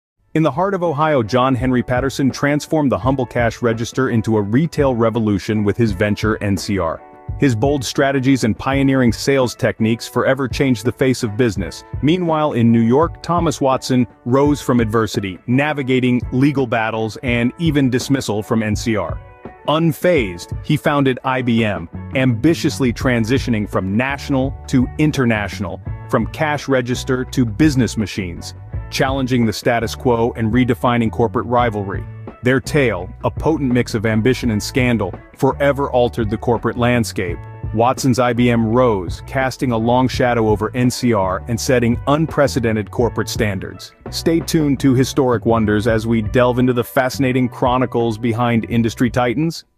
From NCR to IBM: The Dramatic Tale of Sales & Scandal
Explore the riveting history of John Henry Patterson and Thomas Watson with 'History Wonders.'

Discover how Patterson's vision at NCR and Watson's subsequent founding of IBM shaped the course of modern business practices and corporate sales strategies.

This story unveils a legacy of innovation, rivalry, and the evolution of the corporate world, encapsulated in the remarkable journey from the NCR to the global giant IBM.

Reference
*acknowledges NCR is now NCR Voyix but had to keep using former name "NCR" as clarifying old story between NCR and IBM long ago.

Video & Music : KG Studio